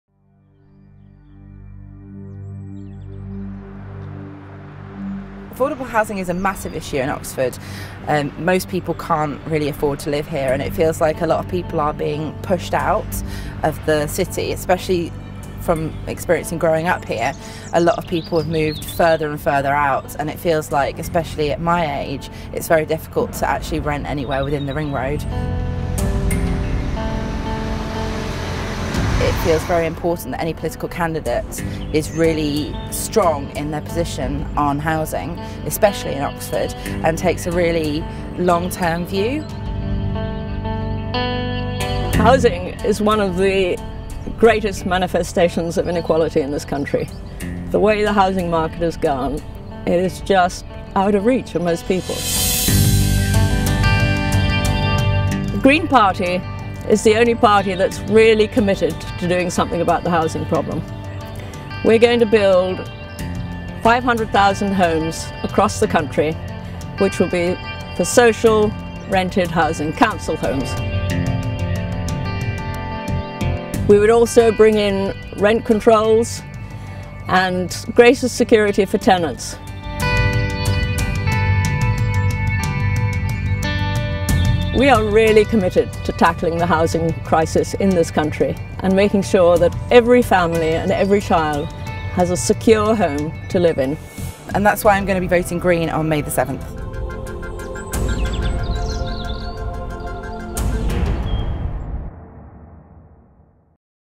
Ann Duncan on the housing crisis and the Green Party
Oxford East Green Party candidate, Ann Duncan, talks about the housing crisis and what the Green Party will do to help solve it. Oxford is the least affordable city in the UK, with houses costing 16 times the average salary - Oxford resident Miranda Shaw says why she'll be voting Green these elections.